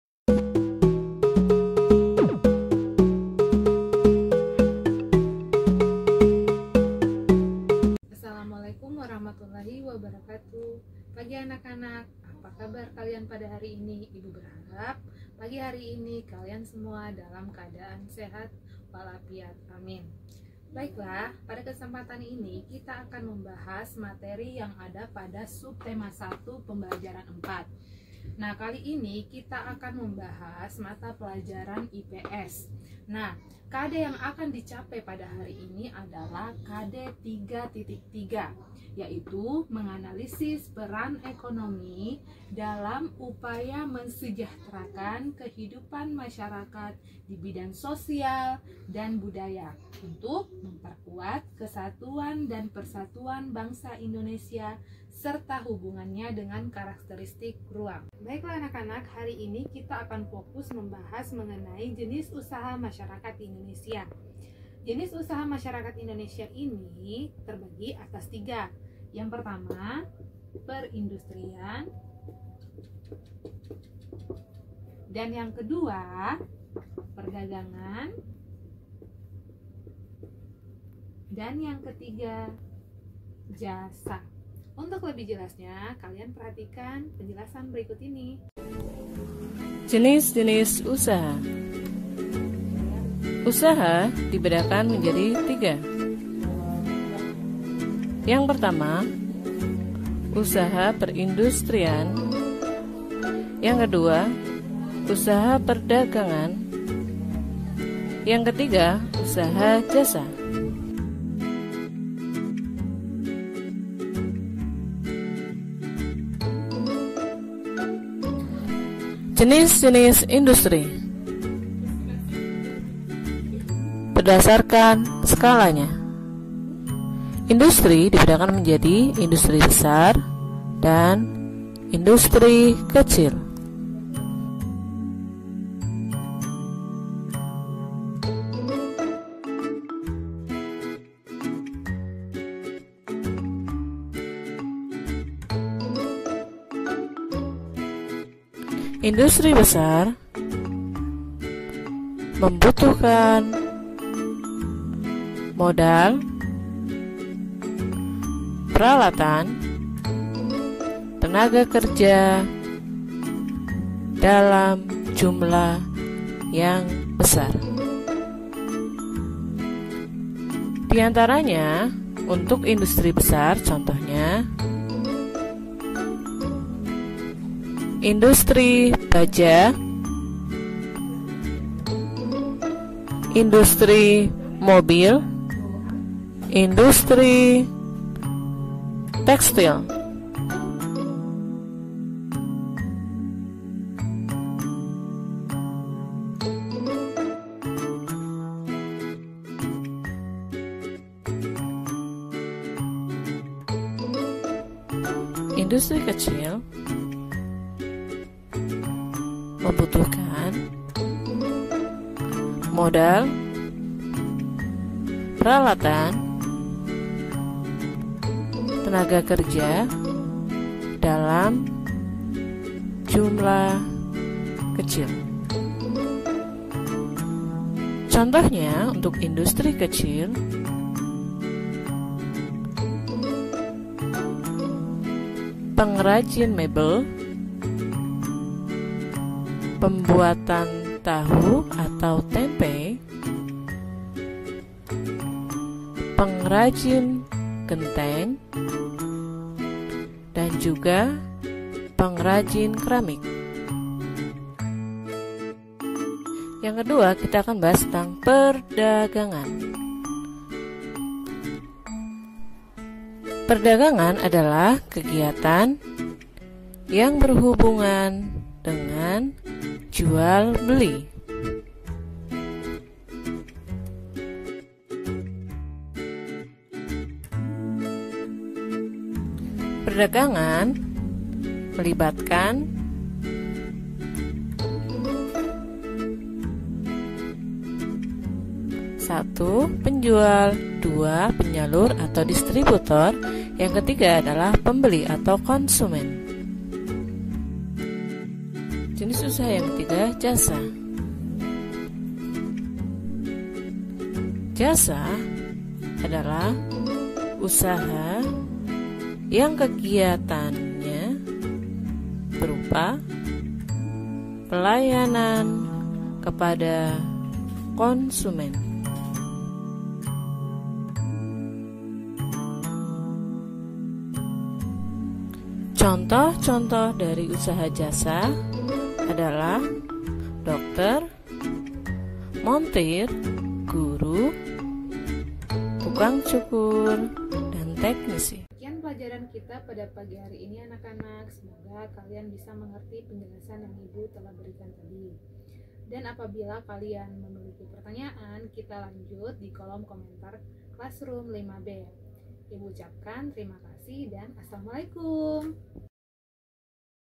Video pembelajaran tema 8 kelas 5
Video pembelajaran dalam rangka belajar dirumah

Materi kelas 5 tema 8 subtema 1 PB.4 tentang jenis usaha masyarakat indonesia.